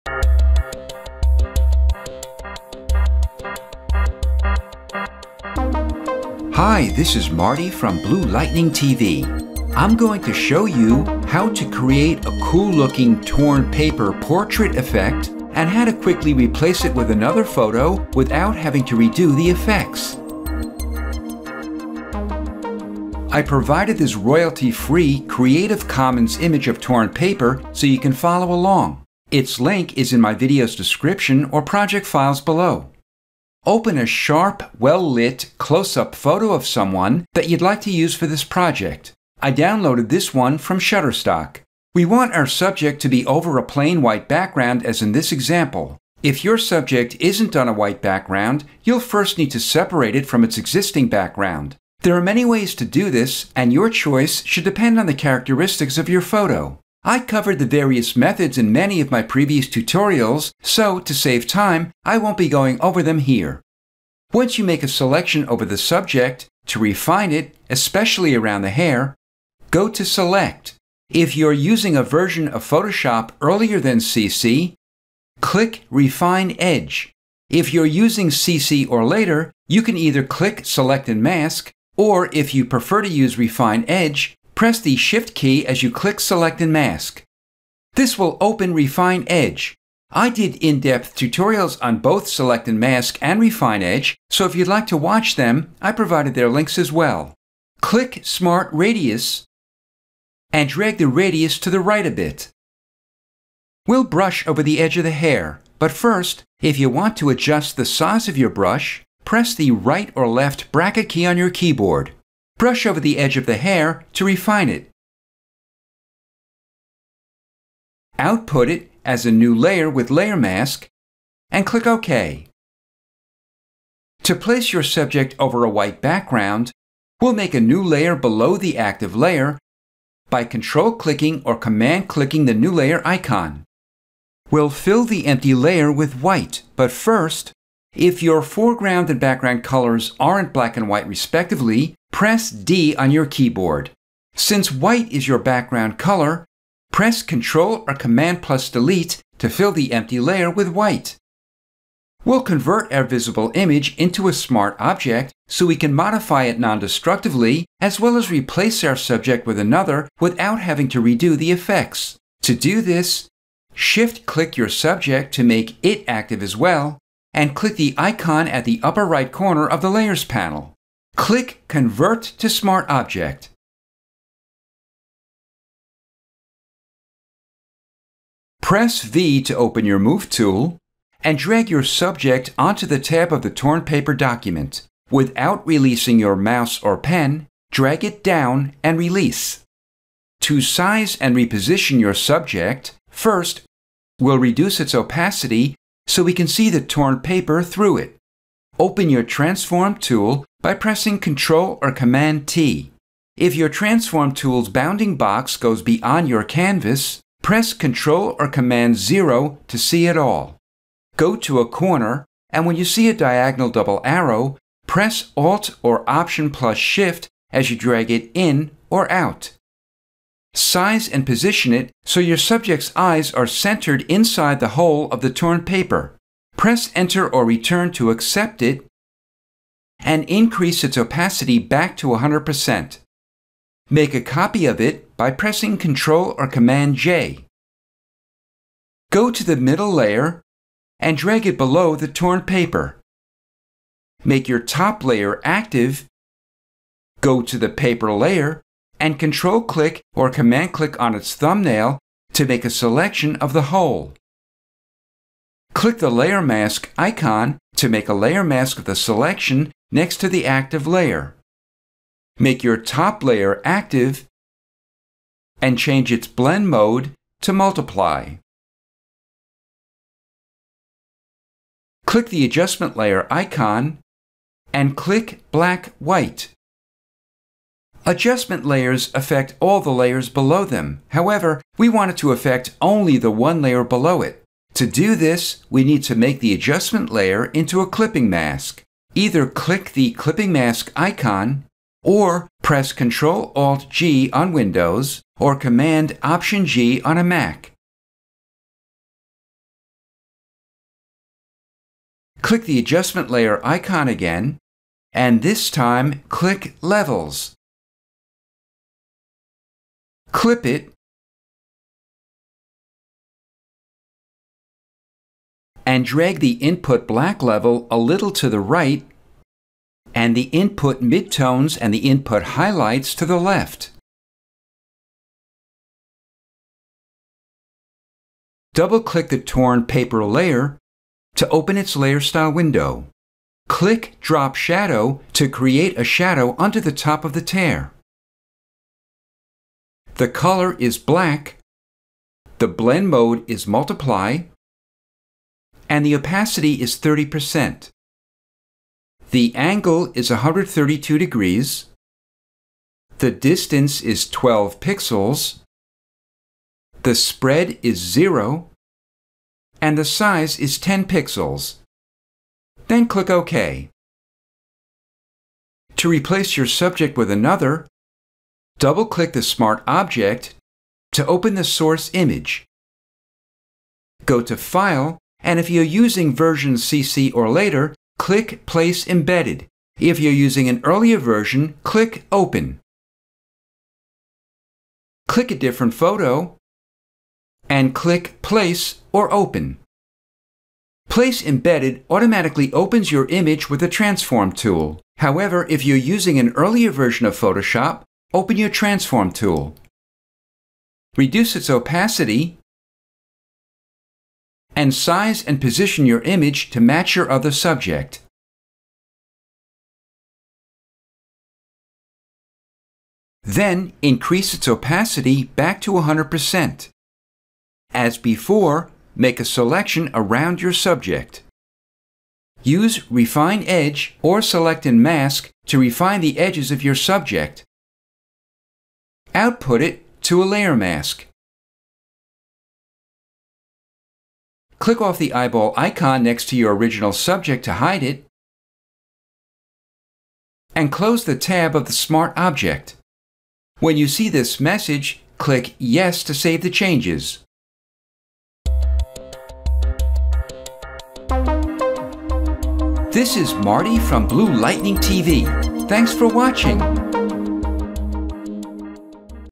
Photoshop Tutorial: How to Quickly Create a Cool-looking, Torn Paper Portrait.
Photoshop CC 2017 tutorial showing how to quickly create a cool-looking, torn paper portrait.

Image IDs:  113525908, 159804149, 94063786